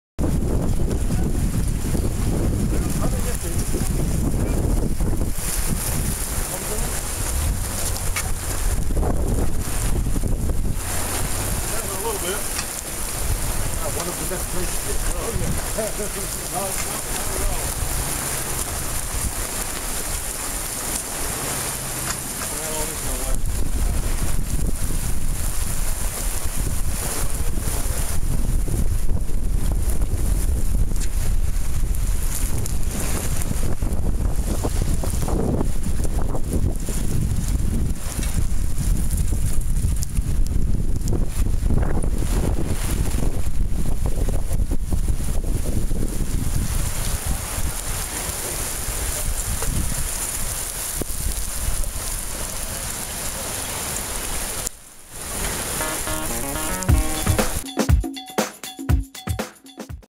Chaparral and Robalo seatrials and the thunderstorm
Well, at Chaparral & Robalo boats, we do it the hard way - taking on trying boats in the harshest conditions and making the most out of it. :-)
It was a fun week trying boats. The weather was not always like this.